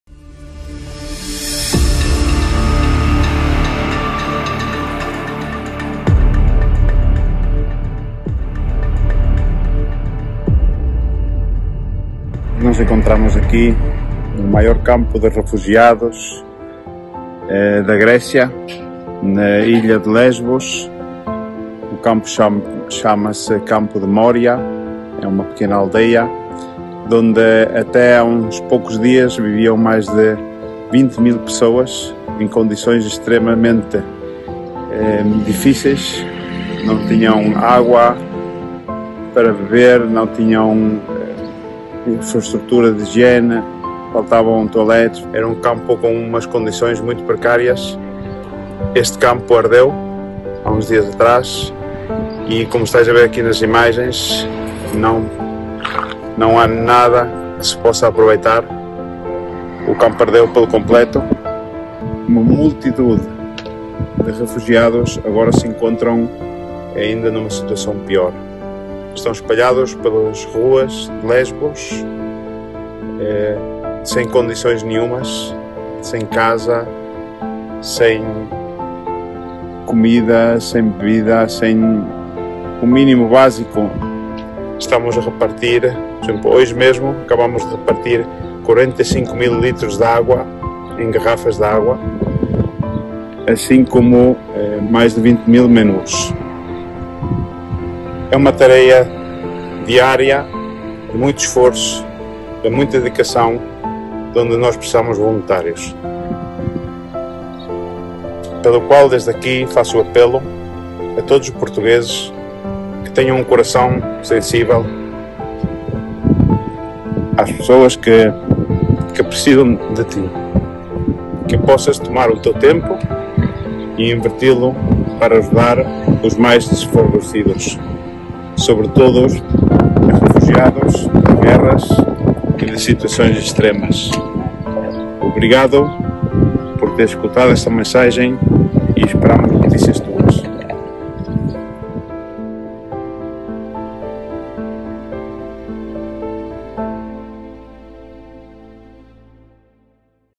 Acampamento de Refugiados - Paulo Oliveira
⇢ Neste momento difícil para os mais de 13 mil refugiados, precisamos da sua ajuda.